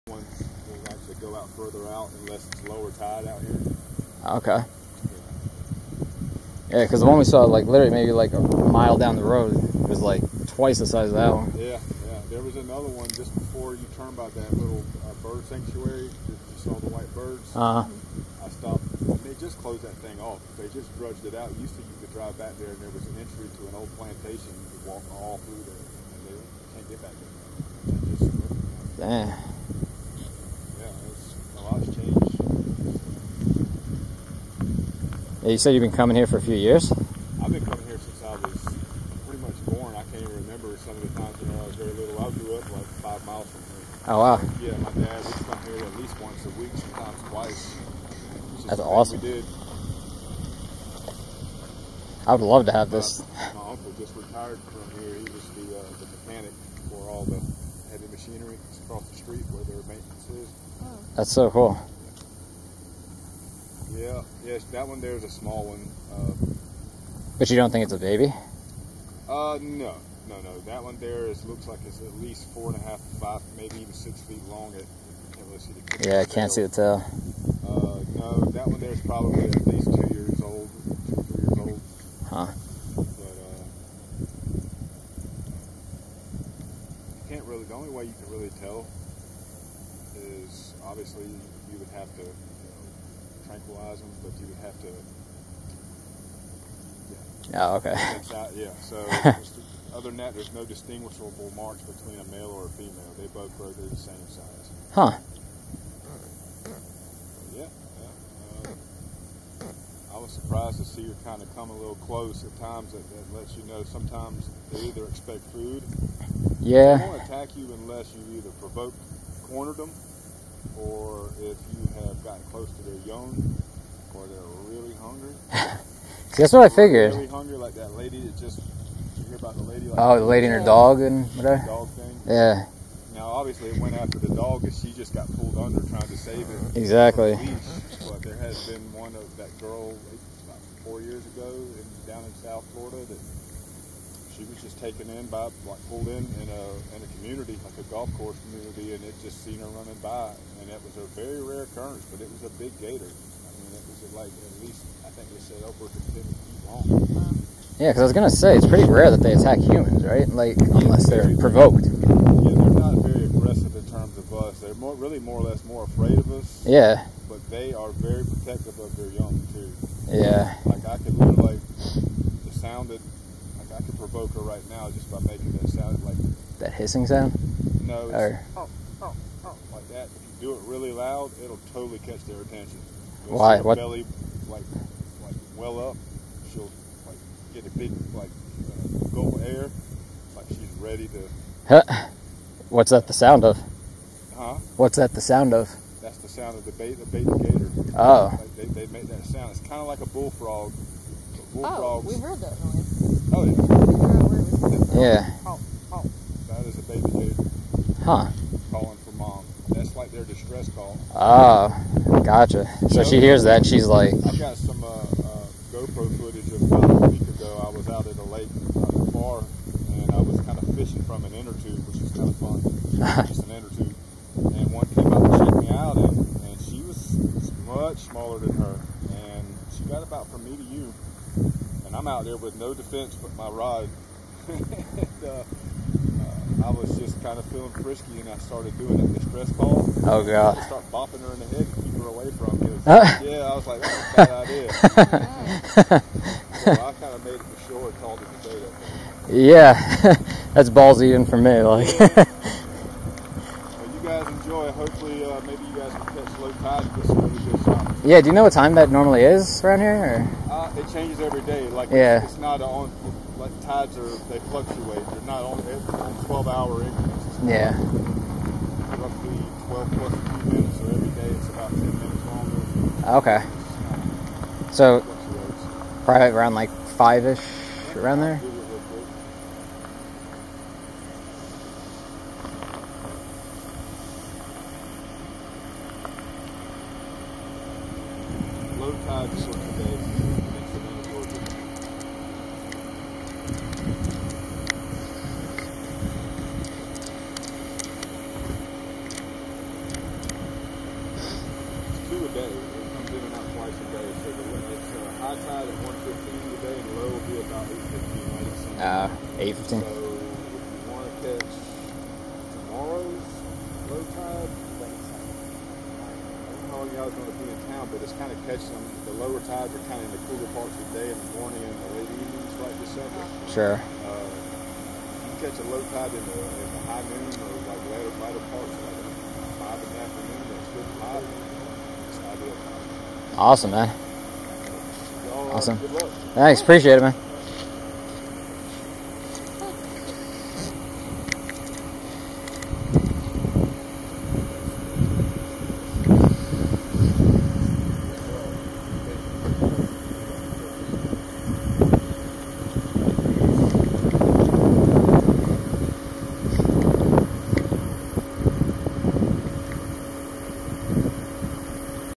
Gator Talk (South Carolina) 👨🏻‍🌾*Will the gator eat me???*🐊
Got some cool close-up shots of a gator down in SC while having an interesting talk with a local about the gators in the area and cool little facts about gators in general and whether this one will actually ATTACK me or not!

*Sorry about the squeaky and noisy camera!*